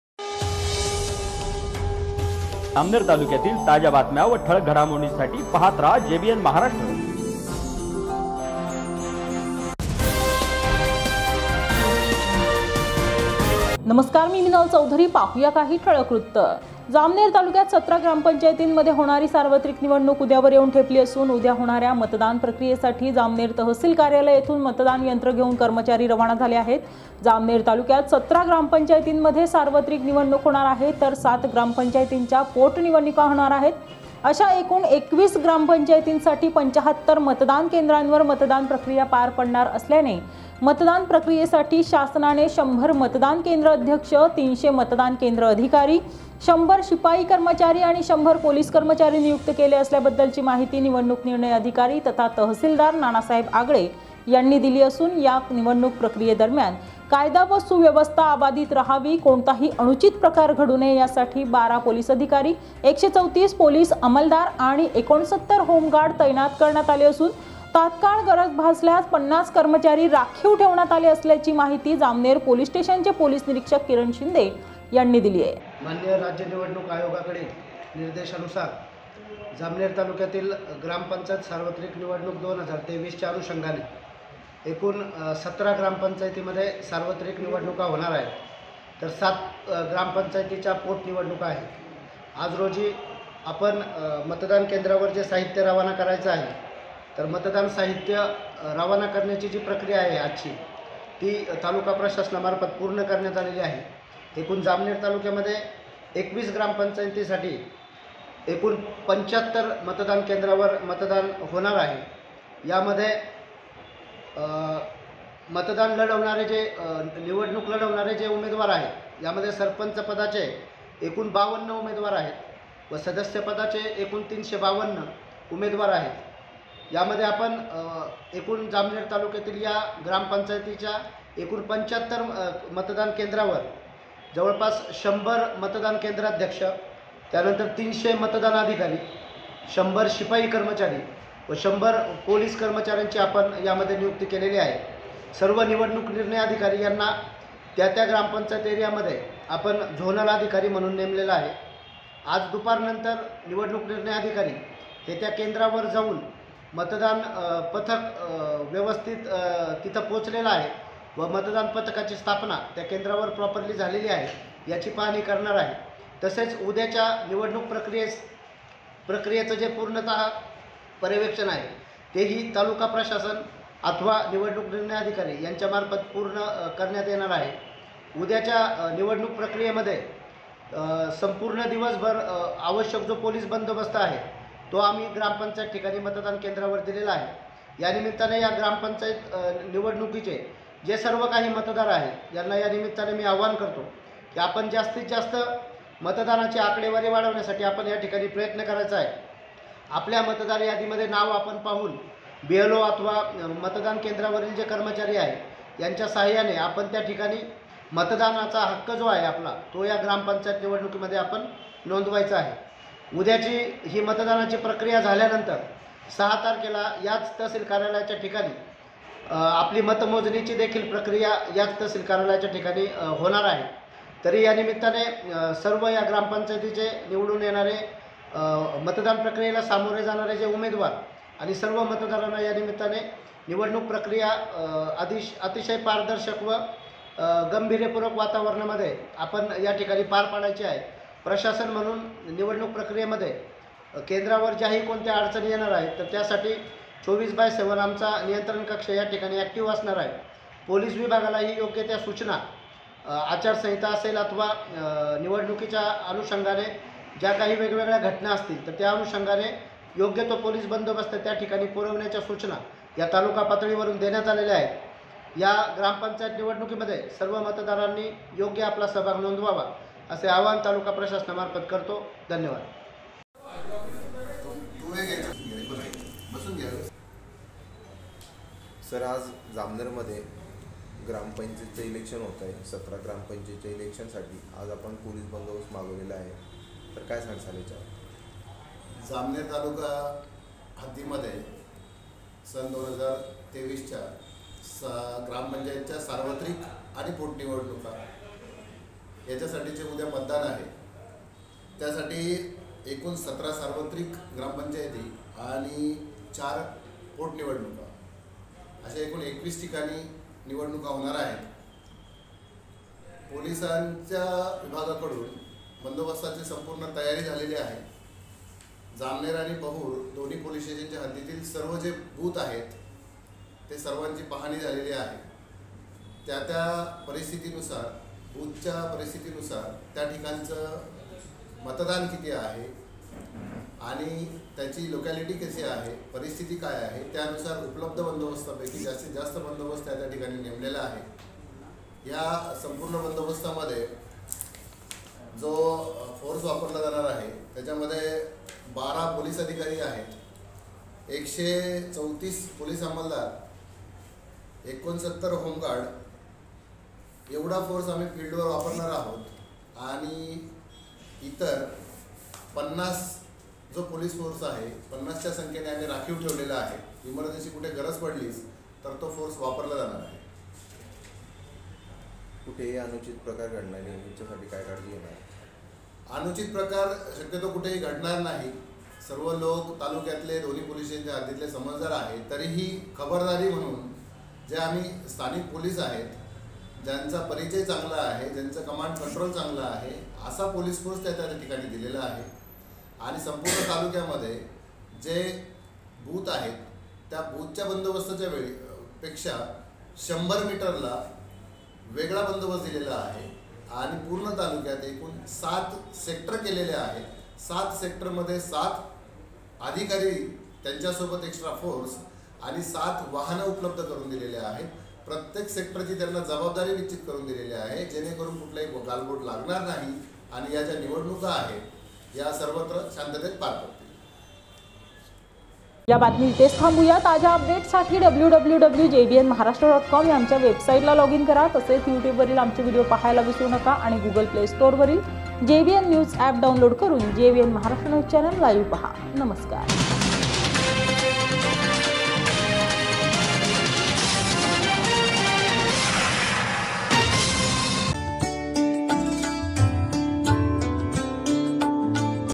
ग्रामपंचायत निवडणूक मतदान प्रक्रियेसाठी तहसील कार्यालय येथून मतदान यंत्र घेऊन कर्मचारी रवाना
ग्रामपंचायत सार्वत्रिक निवडणूक मतदान प्रक्रियेसाठी तहसील कार्यालय येथून मतदान यंत्र घेऊन कर्मचारी रवाना